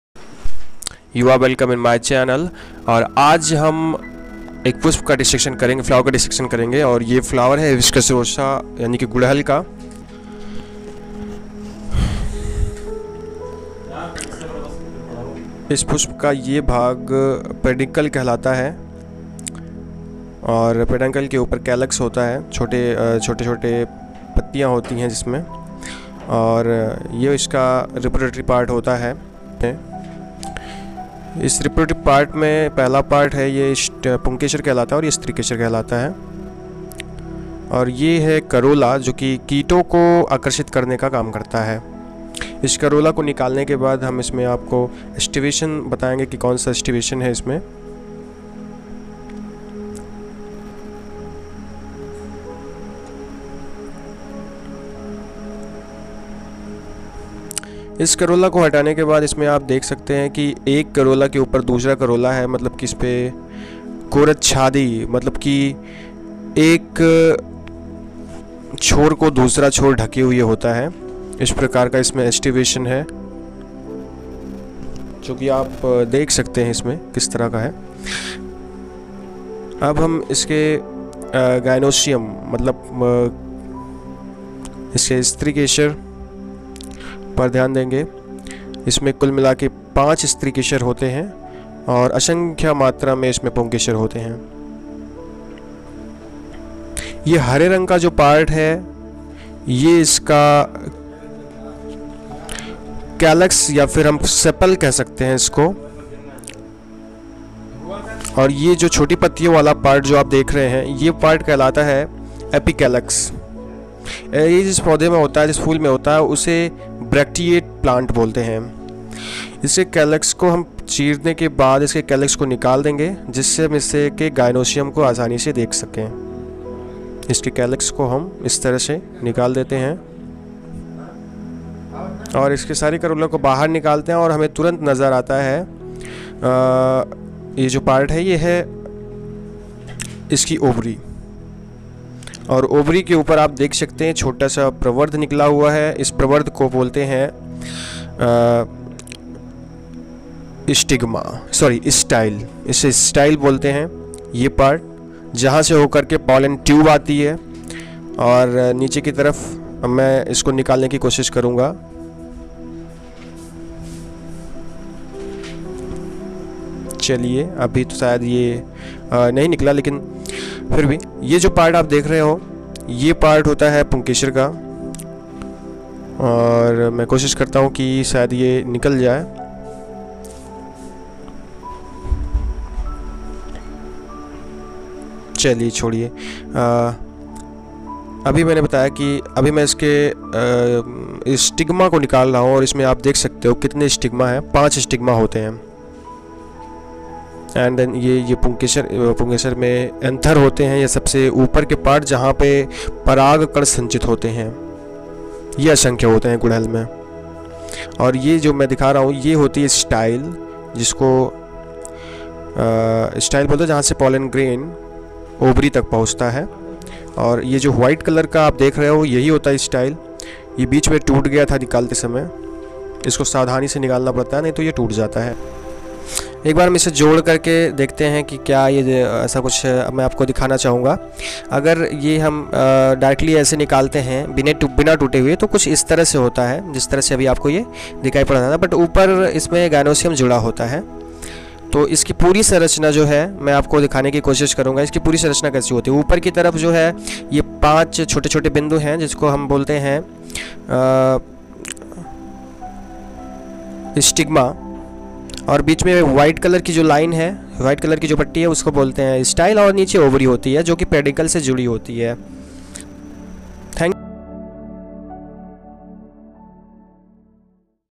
Part of flowers
Is video me flower ka disection krke uske parts ko dikhaya gya hai